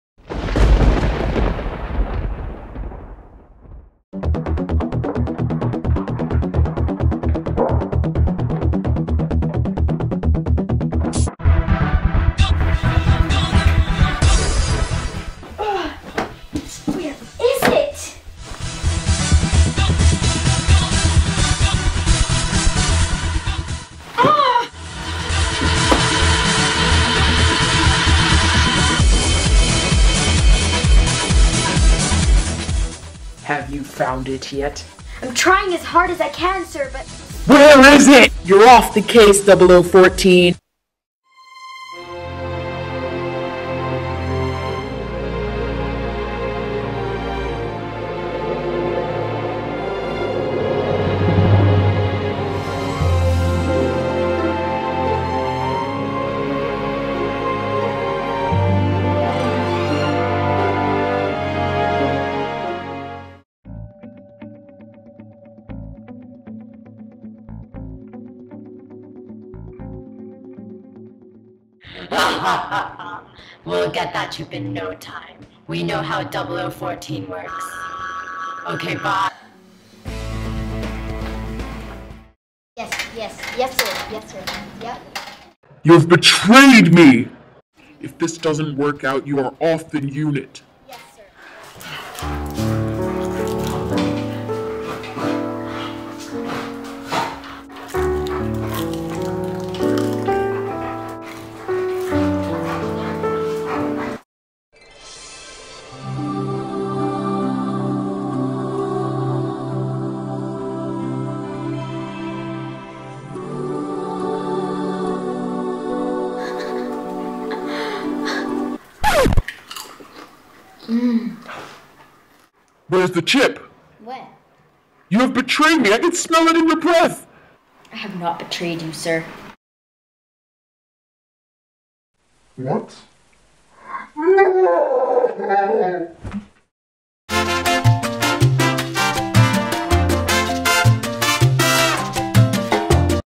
Double 00 Fourteen
The time has come. for the new funny, action-packed AND emotional movie: Double 00 Fourteen. Agent 0014 (played by June Chan) is kicked off the case as she lacks efficiency to find 'the chip'. What will happen next? Will she break down and lose her faith in everything? Or will she stand up and fight back and do her best to retrieve that chip? Find out in Double 00 Fourteen.